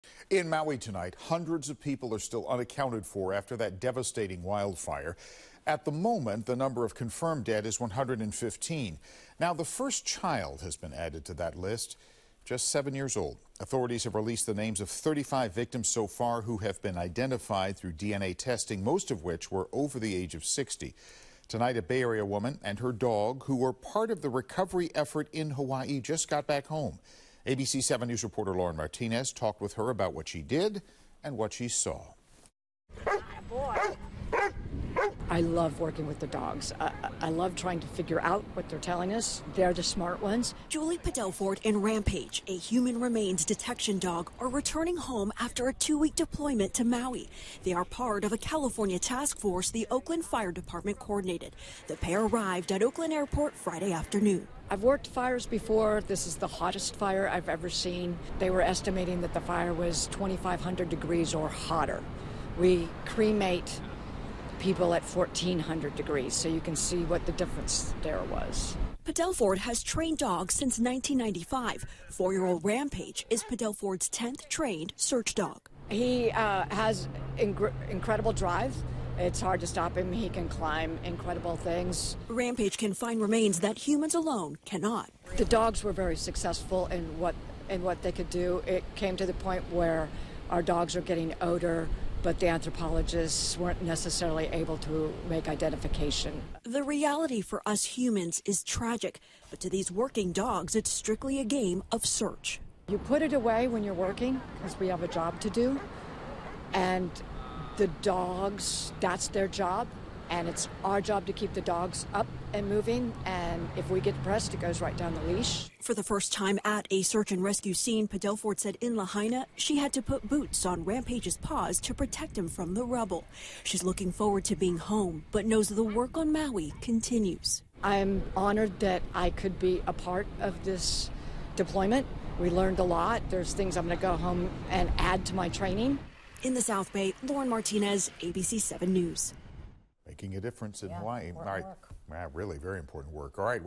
K-9 Detection Dog Returns To The Bay Area From Maui Wildfire Aftermath 8-26-23
The reality for us humans is tragic, but to these working dogs- it's strictly a game of search. Lauren Martinez reports.